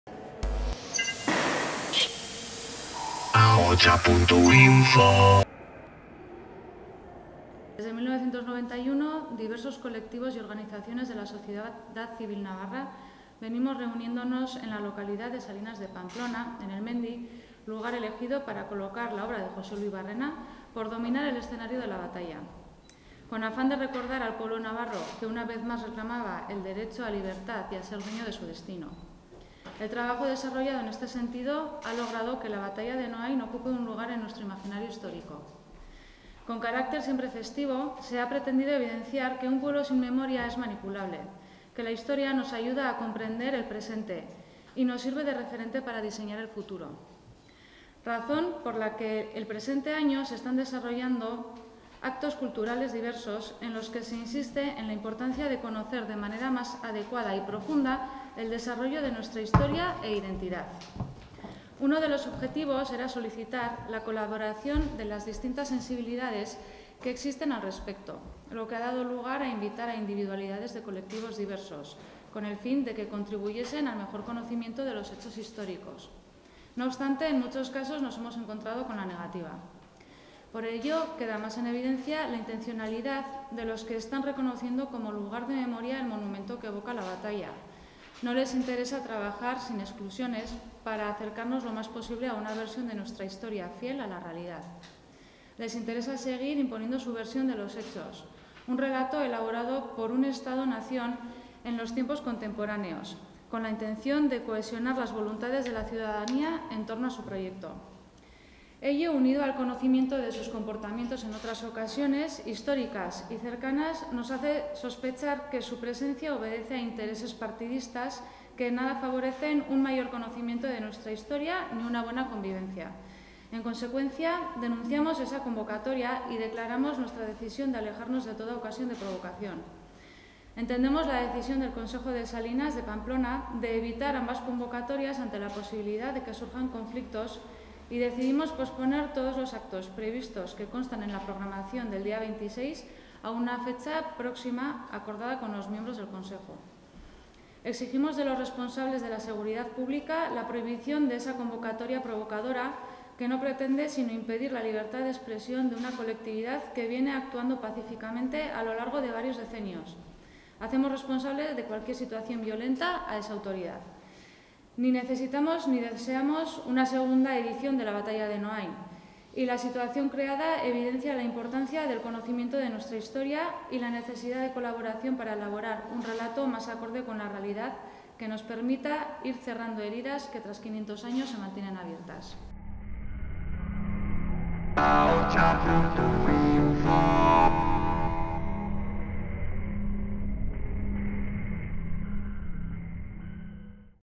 Posponen los actos conmemorativos de la Batalla de Noain y piden prohibir la provocación de VOX
La asociación Batalla de Noain, organizadora de los actos conmemorativos del 500º aniversario de la batalla en la que los soberanistas navarros fueron derrotados por las tropas castellanas, han decidido atrasar a una fecha aun por determinar la convocatoria de este sábado tras conocerse que VOX ha convocado un acto en la misma fecha, hora y lugar con un objetivo totalmente contrario: celebrar la derrota navarra. Desde Batalla de Noain han pedido que se prohíba esta provocación.